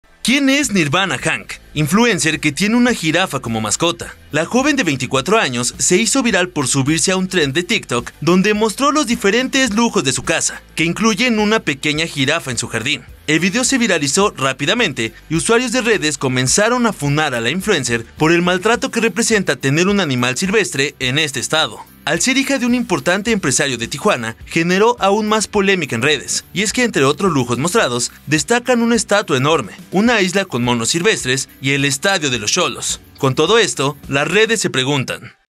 ¿Quién es Nirvana Hank?
Actualmente, la tijuanense se desempeña como socialité, creadora de contenido y conferencista. En sus charlas, Nirvana Hank habla sobre cómo venció al alcoholismo.